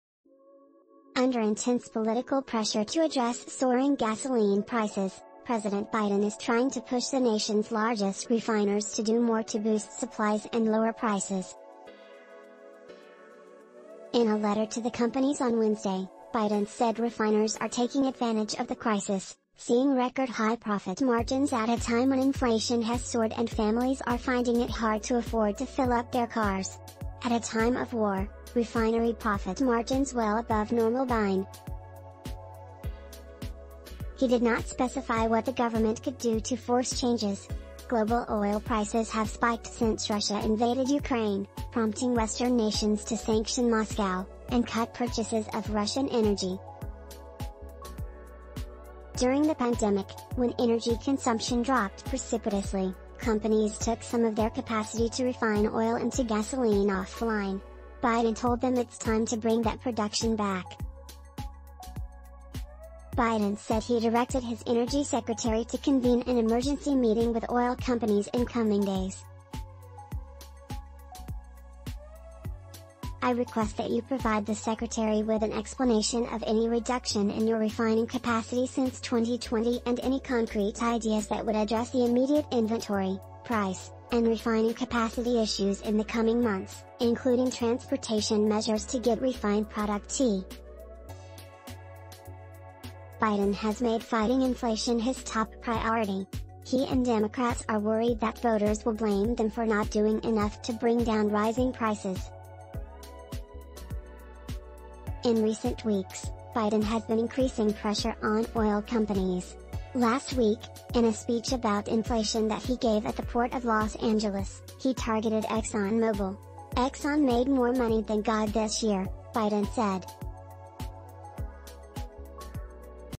Biden is under pressure on gas prices So he's putting pressure on oil
Under intense political pressure to address soaring gasoline prices, President Biden is trying to push the nation's largest refiners to do more to boost supplies and lower prices. In a letter to the companies on Wednesday, Biden said refiners are taking advantage of the crisis, seeing record-high profit margins at a time when inflation has soared and families are finding it hard to afford to fill up their cars."At a time of war, refinery profit margins well above normal being passed directly onto American families are not acceptable," he wrote in the letter, which went to Exxon Mobil Corp., BP and Shell, among others."My administration is prepared to use all reasonable and appropriate federal government tools and emergency authorities to increase refinery capacity and output in the near term, and to ensure that every region of this country is appropriately supplied," Biden said, noting he had already invoked his emergency powers in other ways. He did not specify what the government could do to force changes. Global oil prices have spiked since Russia invaded Ukraine, prompting Western nations to sanction Moscow, and cut purchases of Russian energy. During the pandemic, when energy consumption dropped precipitously, companies took some of their capacity to refine oil into gasoline offline. Biden told them it's time to bring that production back. Biden said he directed his energy secretary to convene an emergency meeting with oil companies in coming days. "I request that you provide the secretary with an explanation of any reduction in your refining capacity since 2020 and any concrete ideas that would address the immediate inventory, price, and refining capacity issues in the coming months — including transportation measures to get refined product to market," he said. Biden has made fighting inflation his top priority. He and Democrats are worried that voters will blame them for not doing enough to bring down rising prices. In recent weeks, Biden has been increasing pressure on oil companies. Last week, in a speech about inflation that he gave at the Port of Los Angeles, he targeted Exxon Mobil."Exxon made more money than God this year," Biden said.